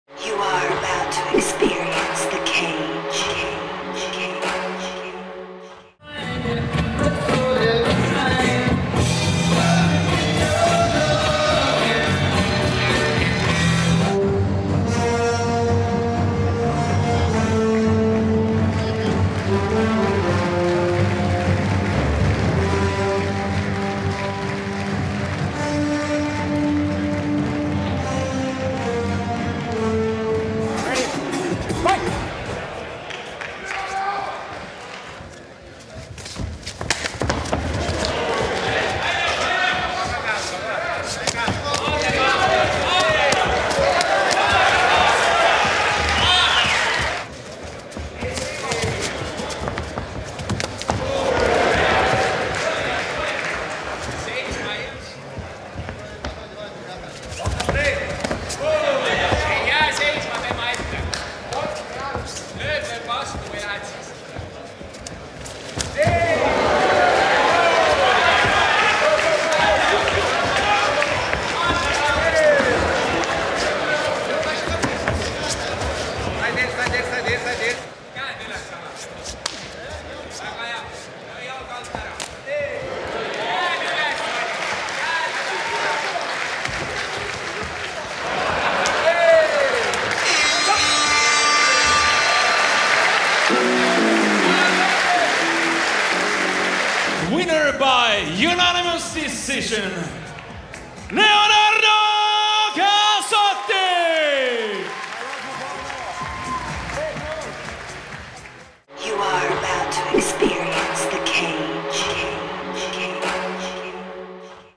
CAGE 48 Leonardo Casotti vs Alar Hutrov Fight Highlight
CAGE 48 Helsinki 7.9.2019
Leonardo Casotti vs Alar Hutrov Fight Highlight
Next: CAGE 49 Helsinki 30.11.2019

CAGE = Mixed Martial Arts Leader of North Europe
CAGE = Vapaaottelua Pohjois-Euroopan huipulla